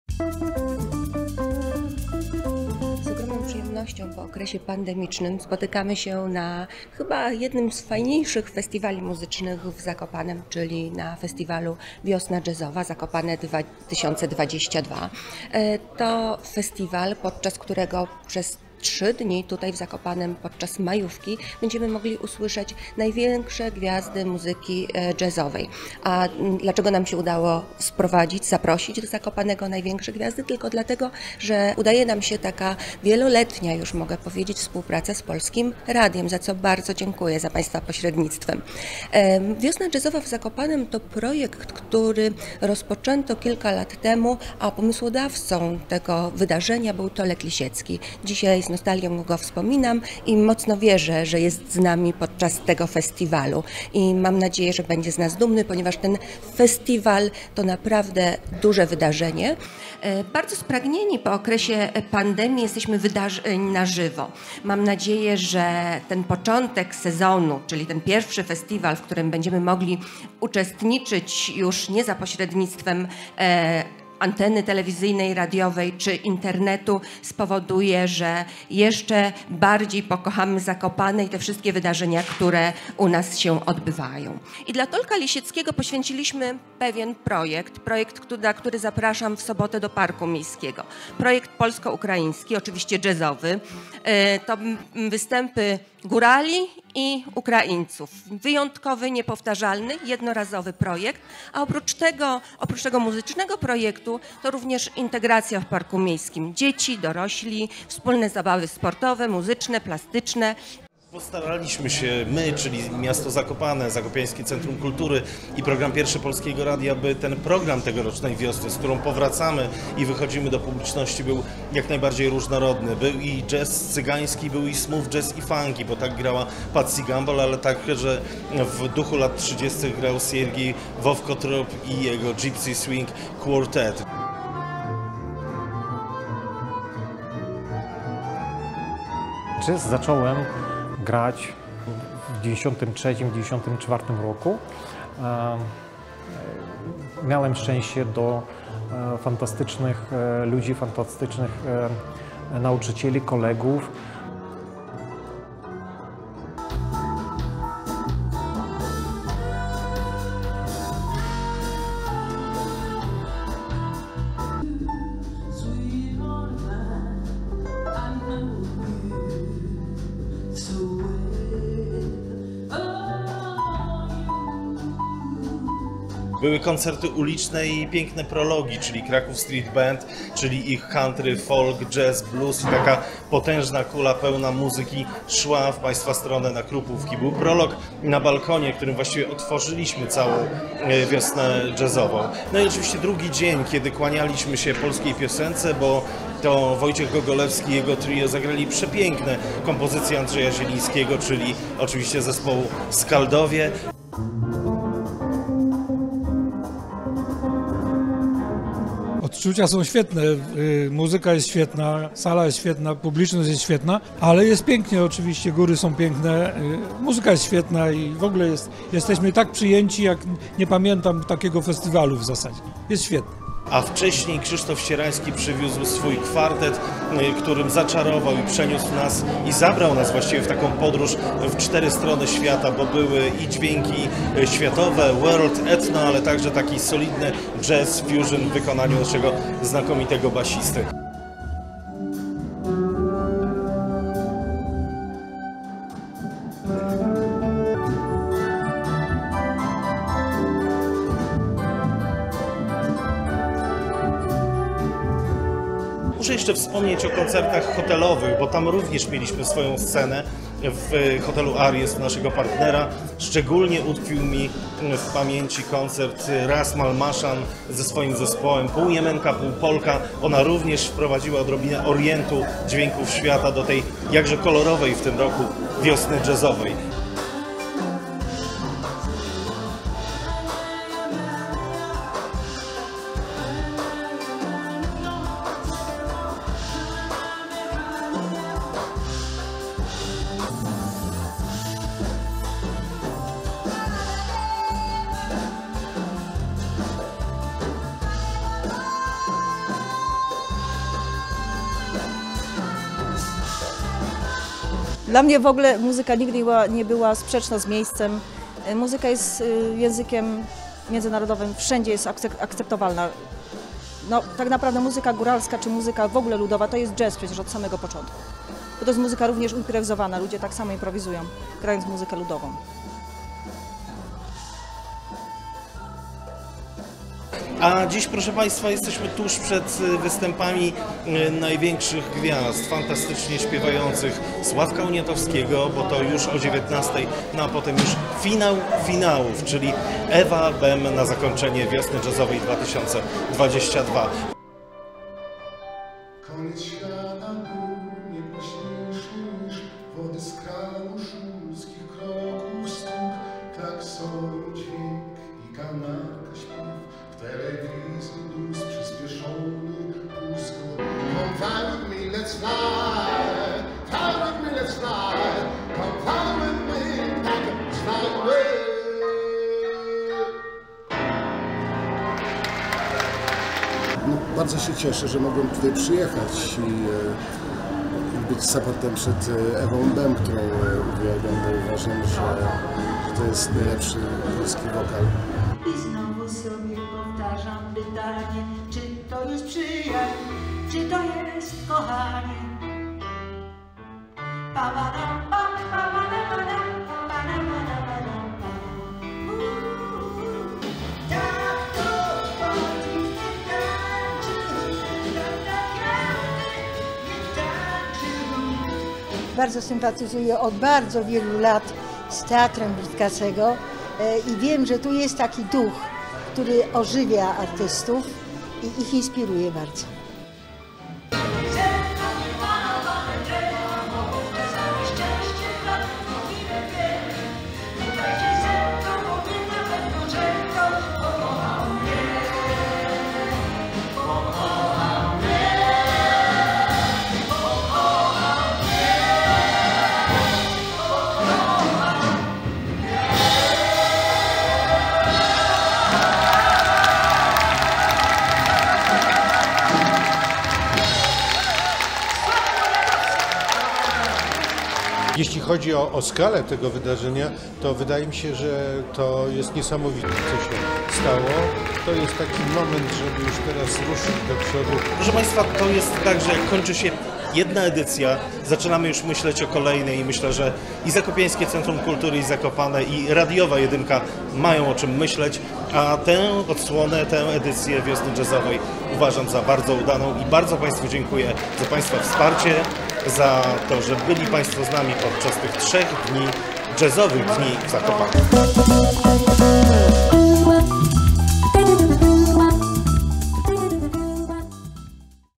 Wiosna Jazzowa w Zakopanem
Wiosna
a jak wiosna to oczywiście jazz w Zakopanem - zapraszamy na krótkie podsumowanie tegorocznej Wiosny Jazzowej w Zakopanem

2018